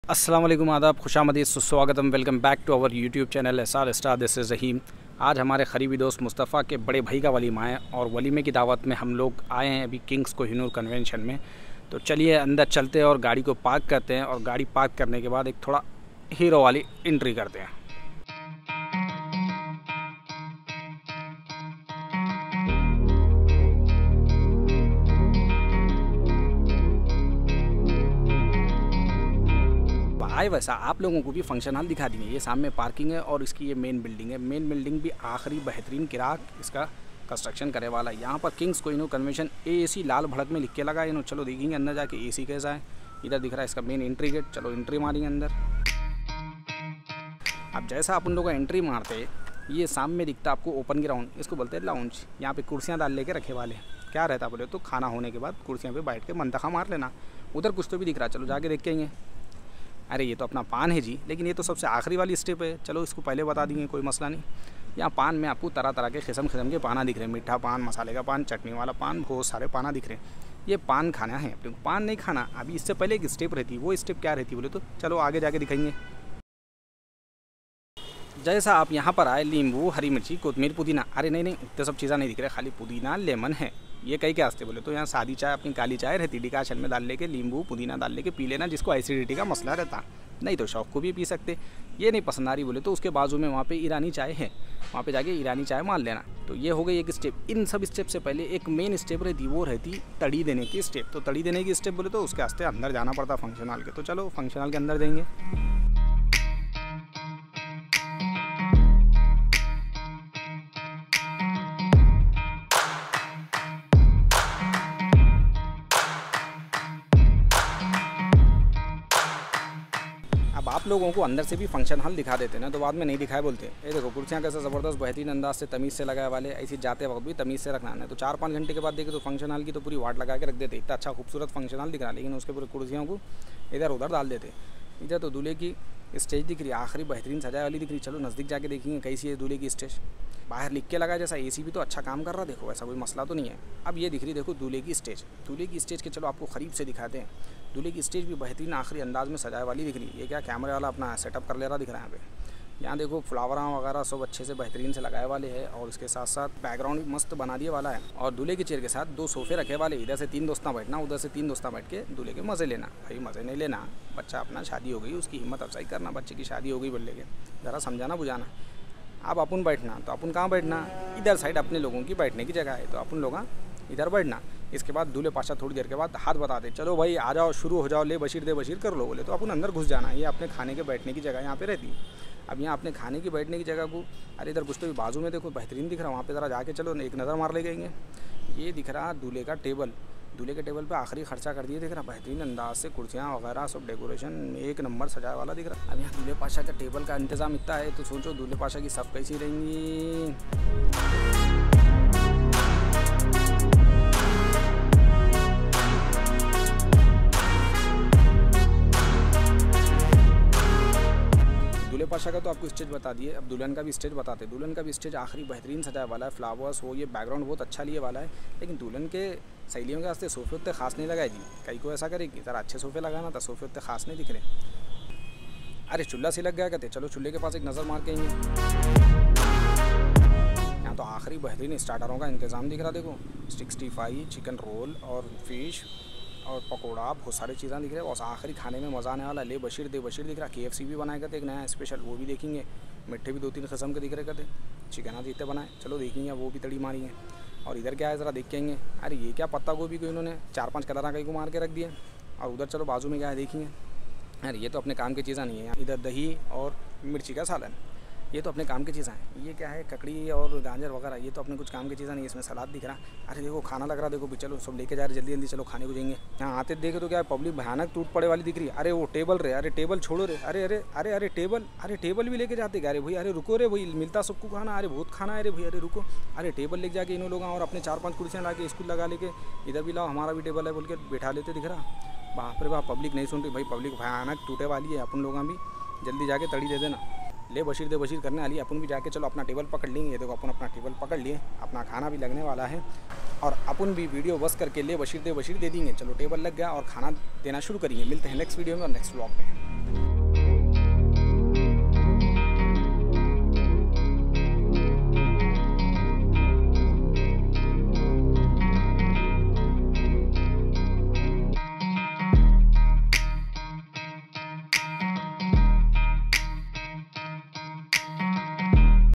Hyderabadi Walima at Kings Kohinoor Convention
Hyderabadi Marriage Reception at Kings Kohinoor Convention Hyderabad.

My Mobile
My Mobile Case
Deity Mic
Boya Mic
Ulanzi Tripod
Action Cam Kit